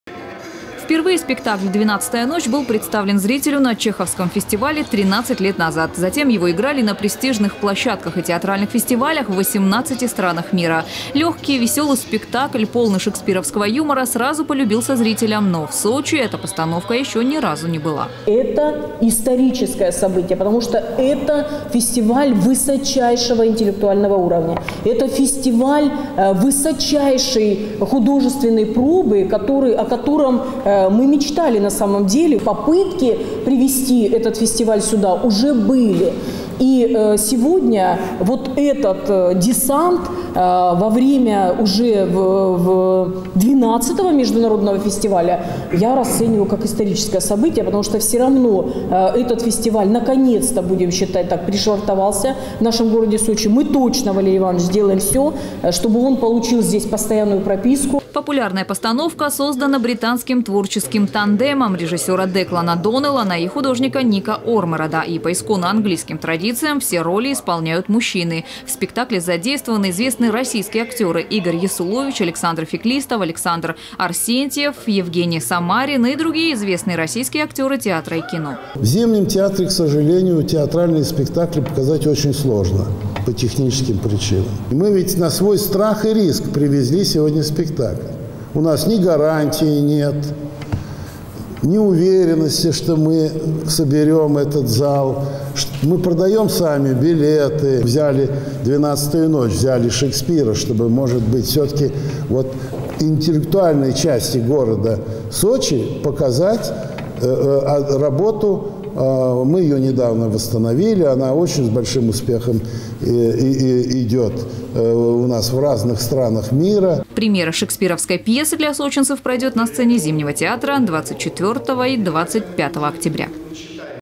Сочинцы увидят "Двенадцатую ночь". Новости Сочи Эфкате
На курорте стартовал Международный театральный фестиваль им.А.П. Чехова. Показ уникального спектакля по пьесе Уильяма Шекспира посвящён Году языка и литературы Великобритании и России 2016 и поддерживается Британским Советом.
Сочи. Новости.
Телеканал Эфкате.